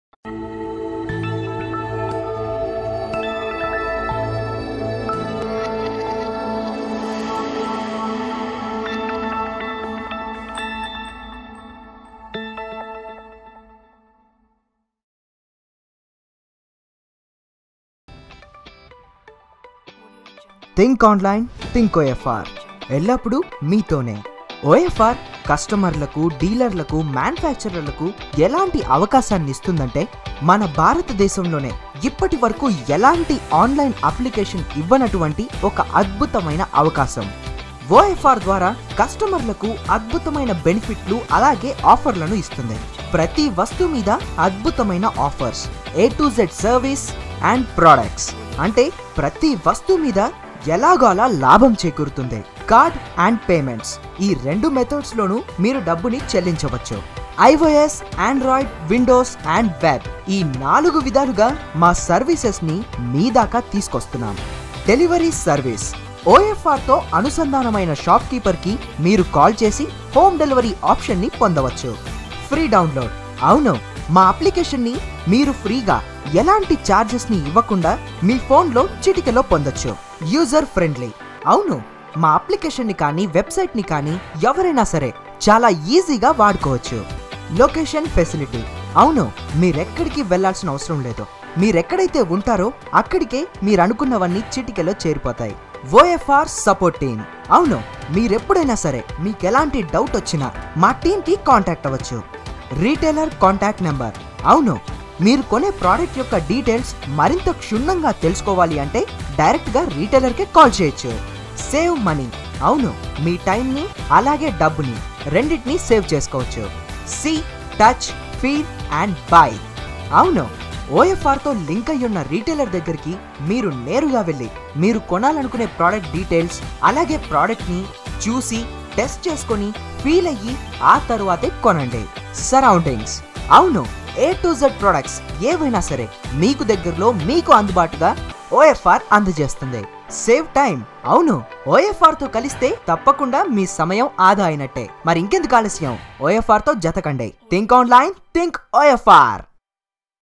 OFR Customer Benefits | Telugu Version
This Video Briefly Describes What Are The Benefits Given To Customers From OFR.

       OFR APP for Customers a unique APP with user friendly E-commerce platform to view all A-Z categories with best quality products at the most affordable prices and offers and to get connected regardless of where you are located with the benefits and features they avail.

   OFR APP వినియోగదారుడు స్నేహపూర్వక ఇ-కామర్స్ ప్లాట్తో ఒక ఏకైక APP అన్ని A-Z వర్గాలను వీక్షించడానికి ఉత్తమ ధర ఉత్పత్తులతో ఉత్తమ నాణ్యత ఉత్పత్తులతో మరియు ఆఫర్లను వీక్షించడానికి మరియు వారు పొందగలిగే లాభాలు మరియు ఫీచర్లతో సంబంధం లేకుండా సంబంధం లేకుండా కనెక్ట్ చేసుకోవడం.